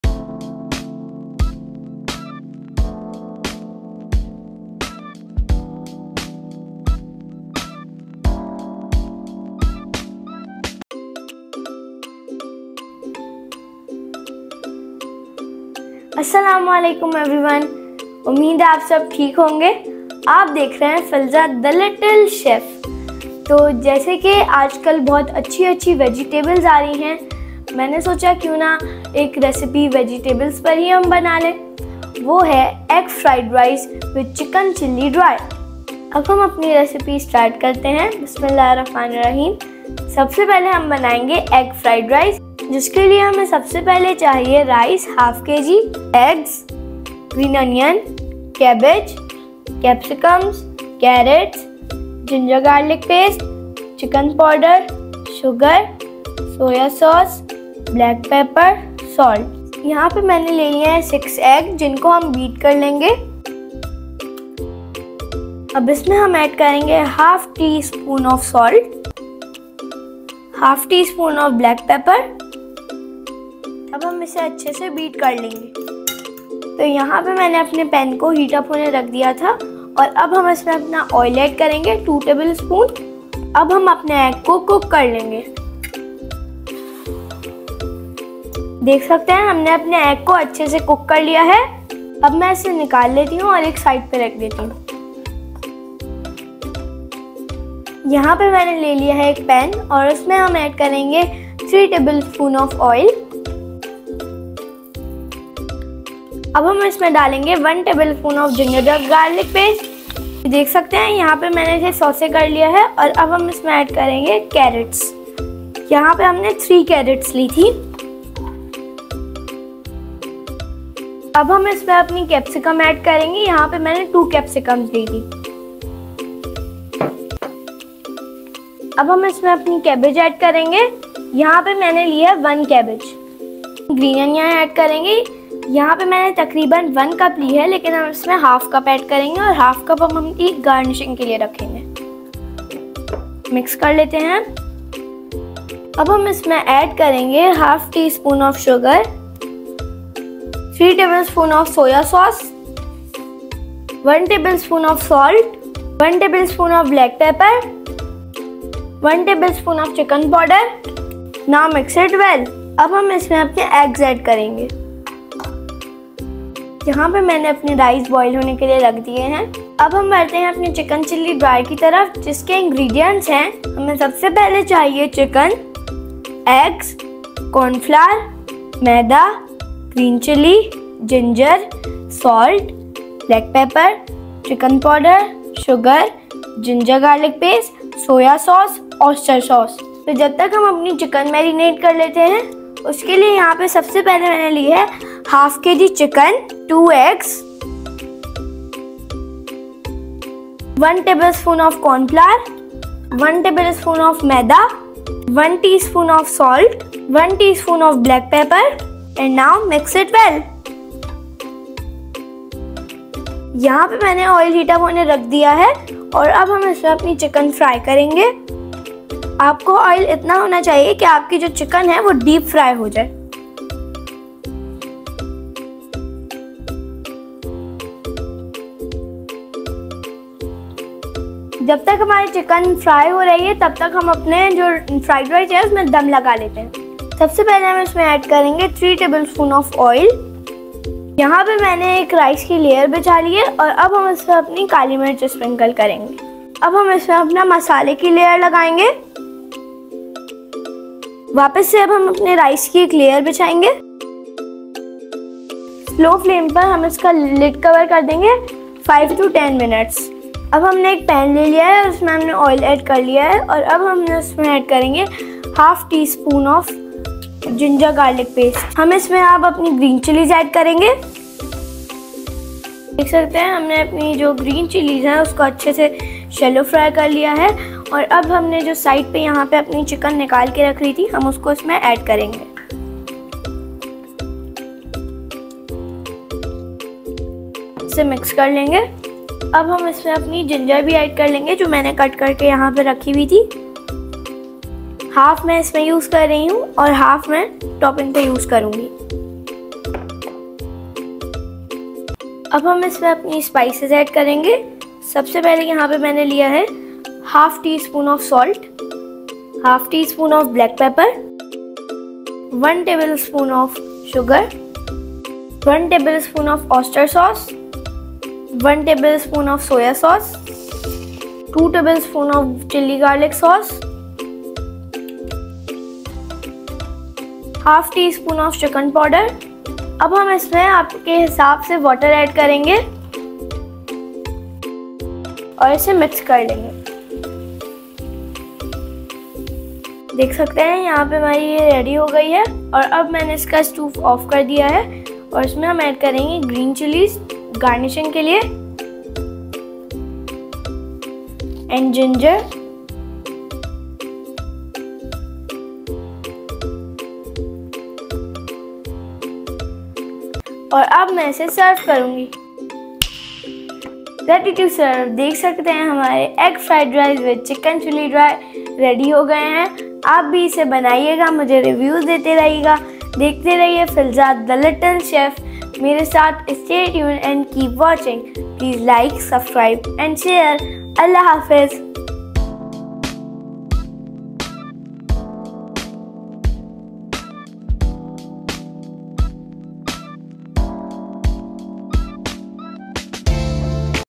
Egg Fried Rice & Chicken Chilli Dry Recipe | Ep 07 | Filza the little chef
Ingredients for egg fried rice:
Rice                               1/2 kg or 500 grams
For egg frying:
Oil                                  2 tbs
Eggs                              6

For vegetables frying:
Carrots                           3
Capsicums                     2
Cabbage                        1 full
Green onions                 1/2 cup
Black pepper                  1 tbs
Chicken powder             1 tbs

Marination for chicken chilli dry:
Chicken boneless          1/2 kb or 500 grams
Eggs                                2
Cornflour                        1 tbs
Maida                              1 tbs

Spices for chicken chilli:
Oyster sauce                1 tbs
Soya sauce                   1 tbs
Chilli garlic sauce         2 tbs
Chicken powder           1/2 tsp
Water
Sliced ginger